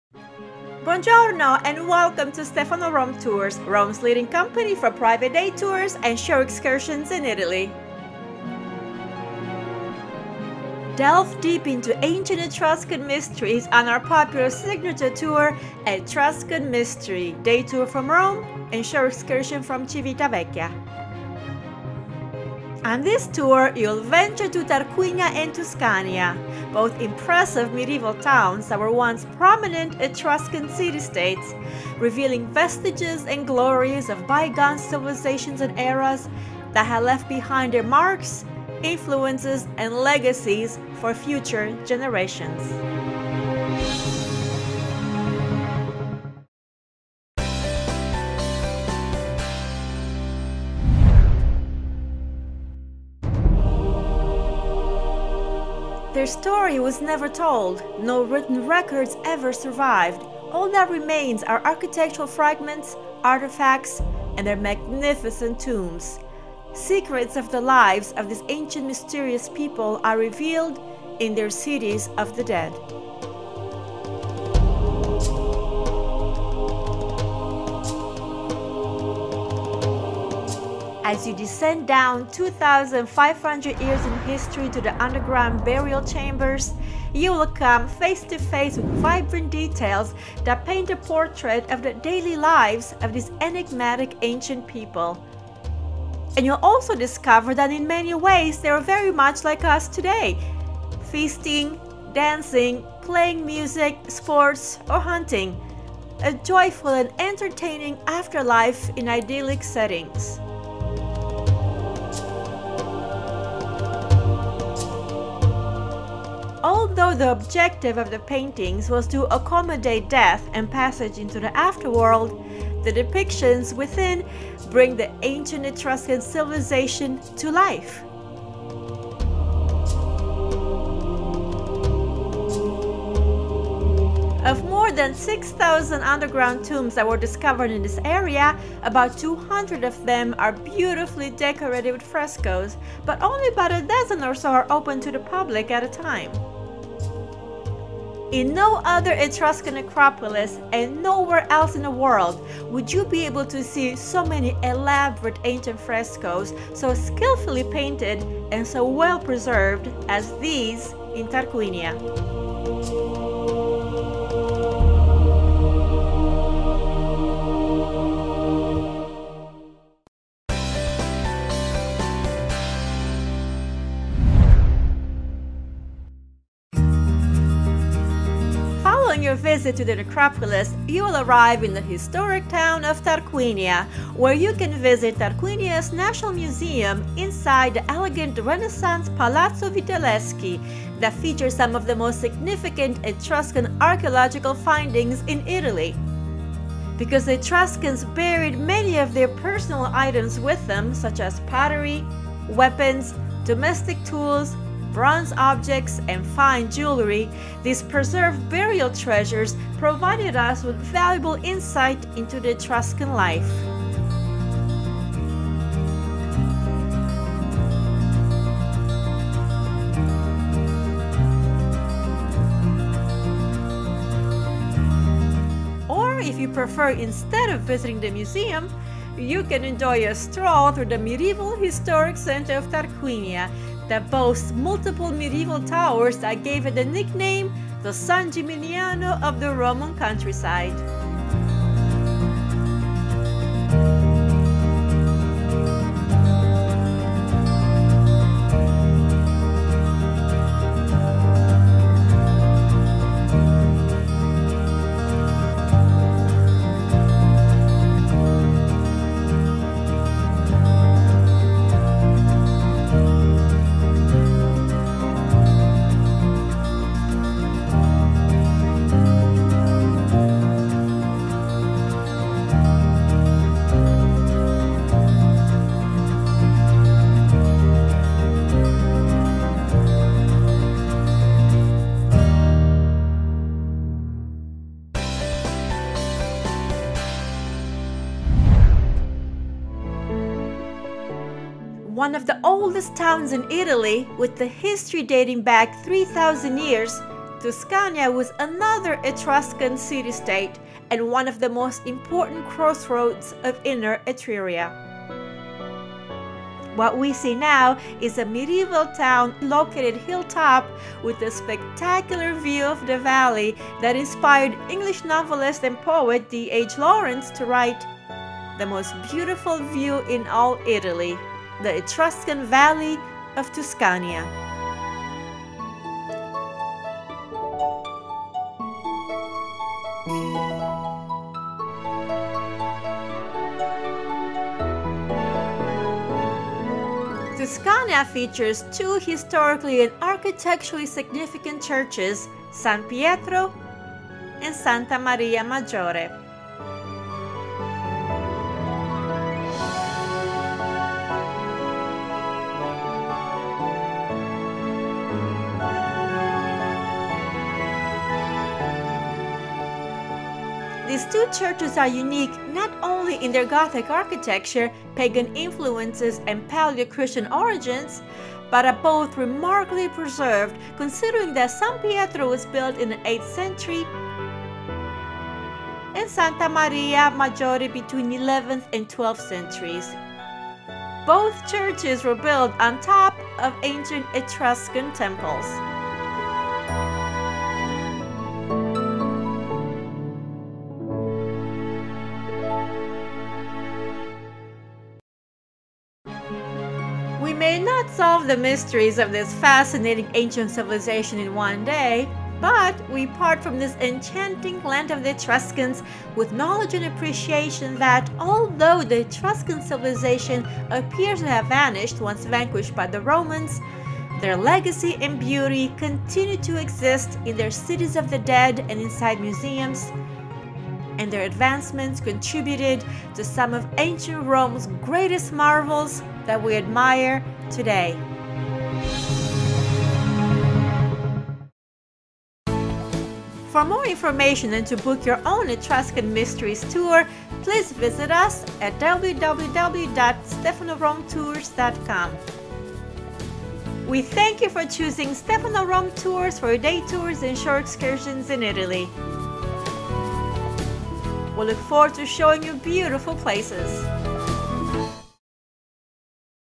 ETRUSCAN MYSTERY (Day Tour to Tarquinia and Tuscania) with Stefano Rome Tours
Our popular signature tour Etruscan Mystery Day Tour from Rome and Shore Excursion from Civitavecchia takes you to the ancient lands of Etruria to explore the famous painted tombs of the Monterozzi Etruscan Necropolis in Tarquinia, enjoy the historic town of Tarquinia, followed by a visit to Tuscania.